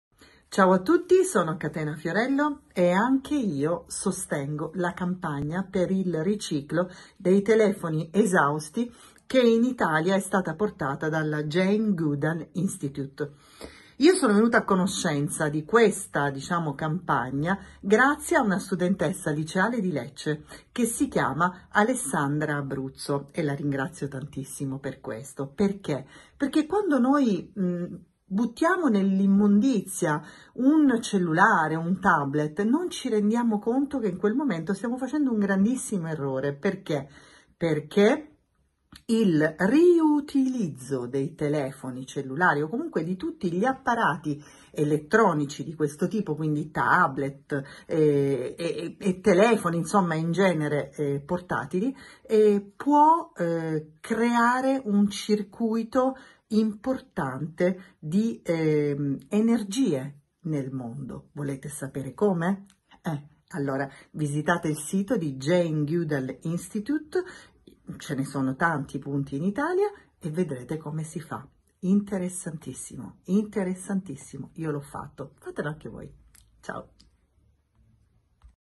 La scrittrice Catena Fiorello per la Campagna riciclo cellulari non utilizzati
La scrittrice Catena Fiorello partecipa attivamente alla campagna del Jane Goodall Institute Italia: alla nostra giovane attivista  del programma Roots&Shoots Alessandra Abruzzo ha già consegnato i suoi vecchi cellulari ed ora ci aiuta con questo bel messaggio a diffondere l’importante iniziativa.

Grazie a Catena Fiorello per il suo appoggio e per la sua sensibilità ambientale. Tutti noi possiamo e dovremmo agire per la sostenibilità, per il nostro pianeta!